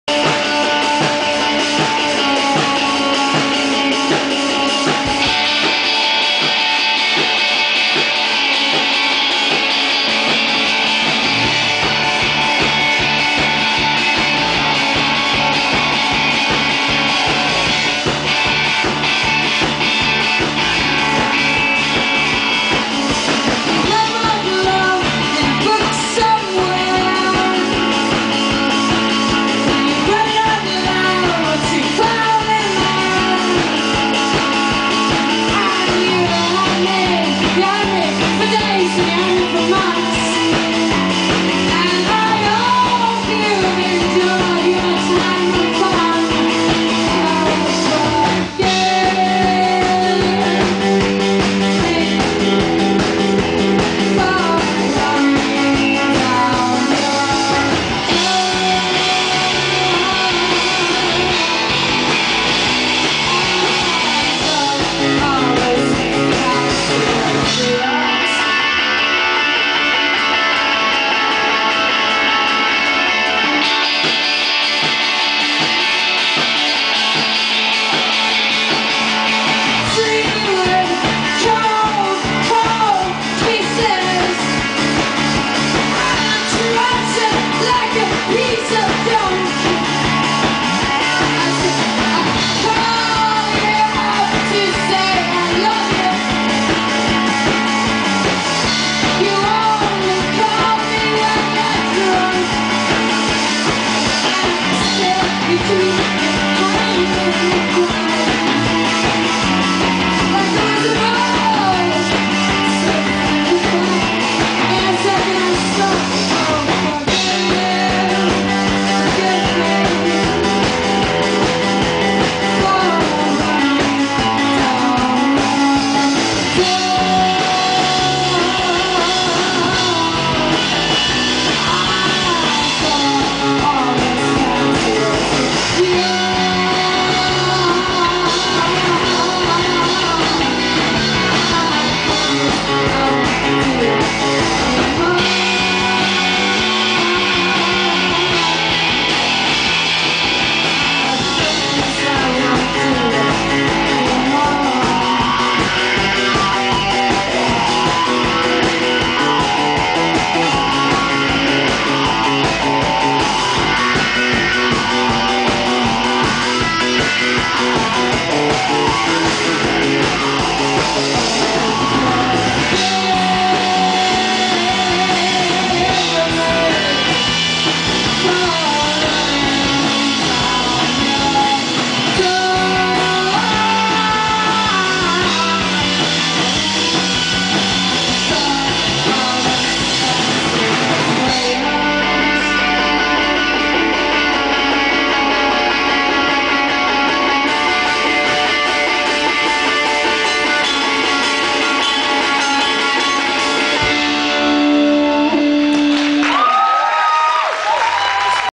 Ida Maria live Ung08 Stockholm
Ida Maria played a short but awesome gig on the small stage in Kungsträdgården at Ung08 festival! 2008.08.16